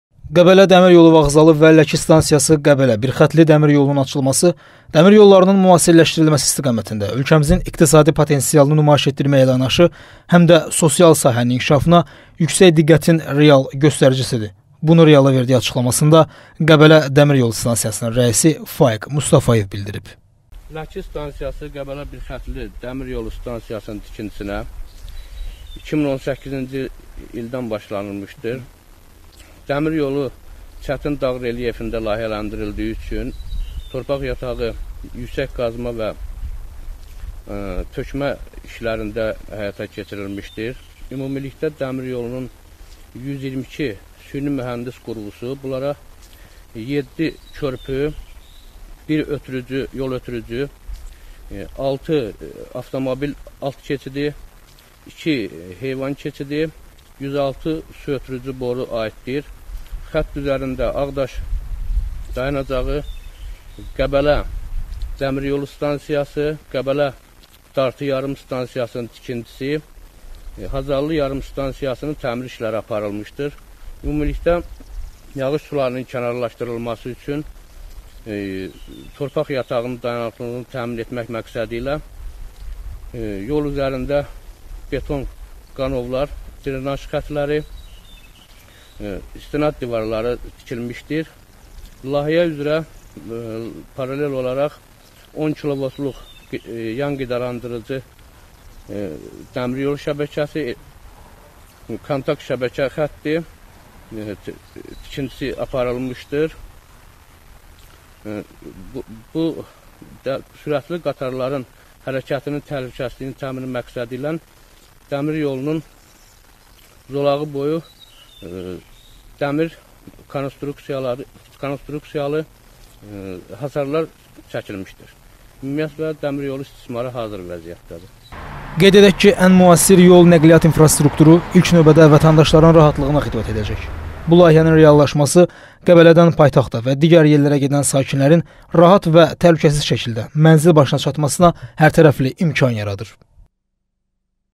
Dəmiryolu vağzalının açılışı olub
Ləki stansiyası-Qəbələ birxətli dəmiryolu da istifadəyə verilib